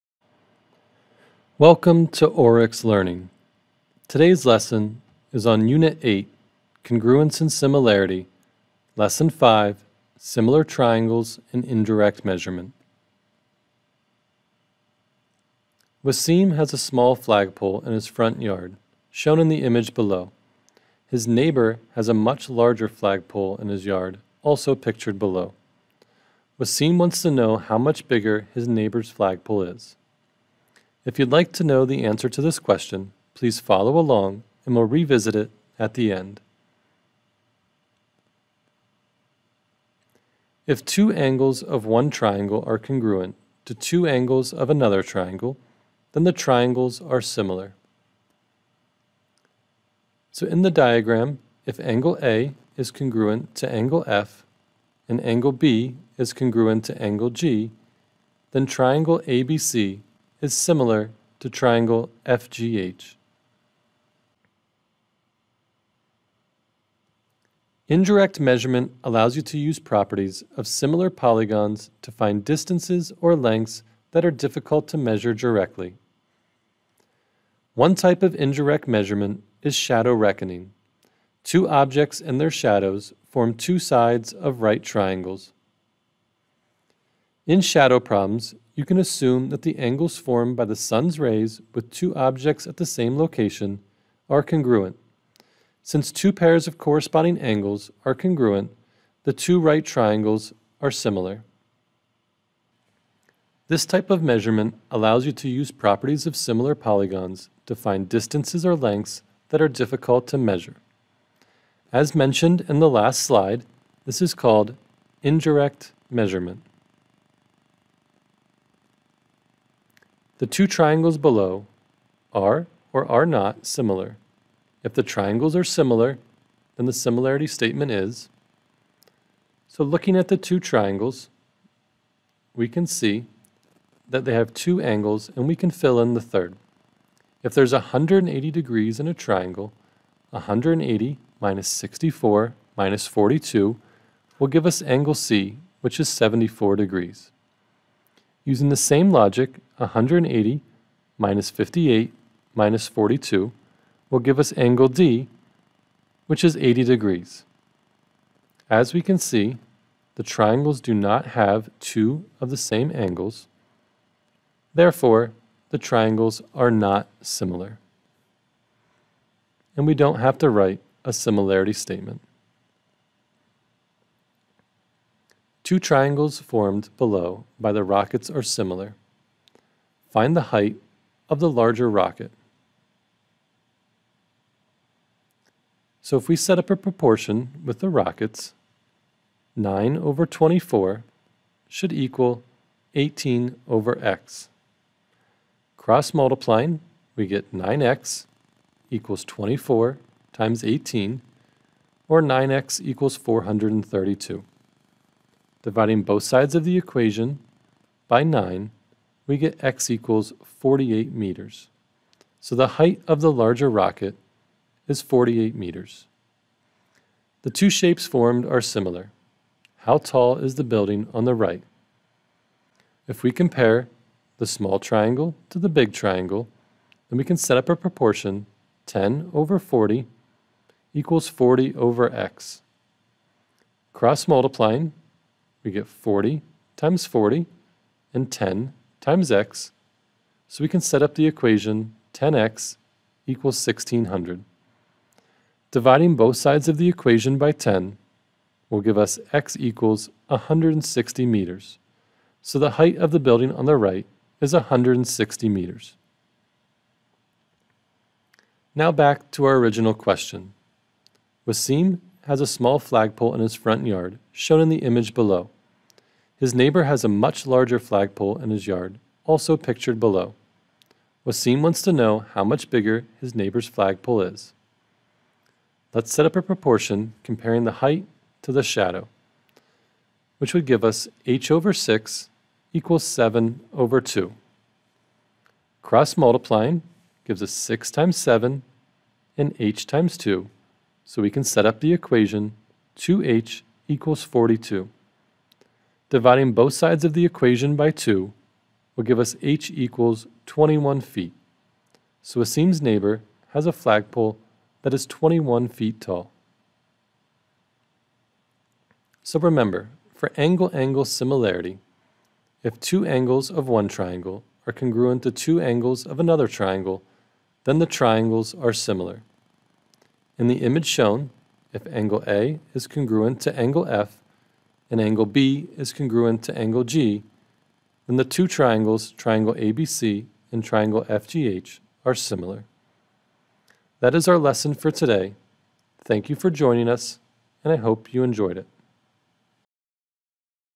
Similar Triangles and Indirect Measurement | Grade 8 Math | Oryx Learning
You will learn how to:
● Identify the basic vocabulary distinguishing between direct and indirect measurement.
● Find similarity statements of similar triangles.
● Find missing side lengths using indirect measurement.
● Solve application questions to find the missing length using indirect measurement.